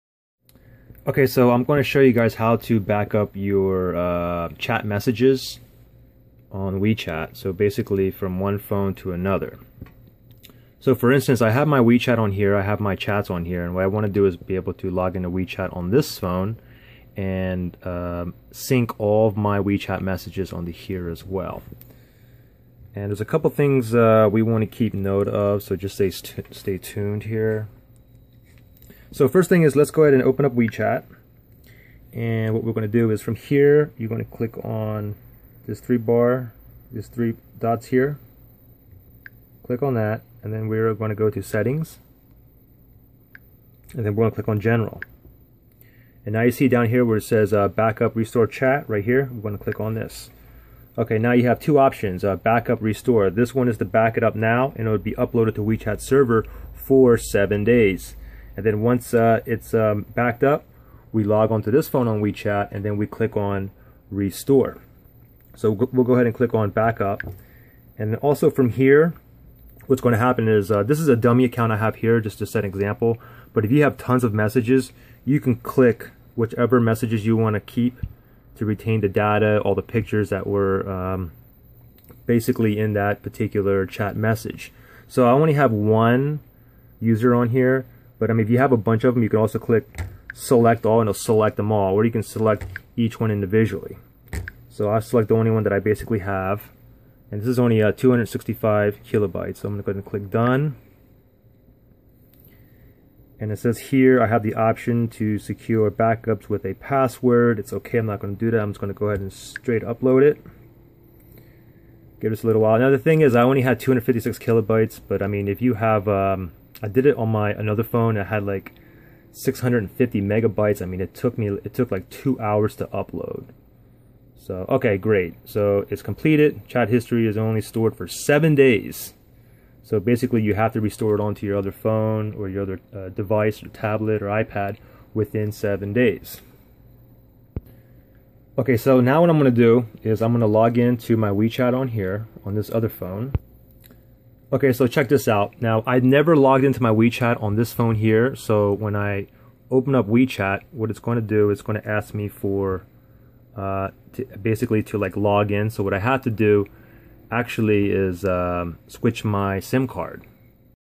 WECHAT: BACKUP & RESTORE CHAT HISTORY MESSAGES EASILY
Note: If this does not work you can try data backup software

You can easily backup and restore your Wechat history from one device to another (eg phone to phone, phone to ipad, phone to computer, ipad to tablet, etc)

Important note: If you get an ERROR while trying to upload or download, just keep pausing and restarting. I had to do it like 40 times and it took me 3 days to do, but all good now.

While on wechat with all your message you want to sync to another device do this:
- on the top right click the 3 vertical dots
-click on general
-then click on "Backup/Restore Chat History
- click on backup (once complete you have 7 days to restore (download) the history onto you other device.

-Once backup is complete, log in wechat on your other device
- again click on the 3 vertical dots on the top right
-click on general
- And now you click on "RESTORE"

After restore is complete you should have all your chat history including message, videos, photos, etc

Important note: I recommend everybody to link your Wechat to your email with no delay. This can save you time and headaches later on...trust me!

Subs, likes, comments all appreciated. Thanks.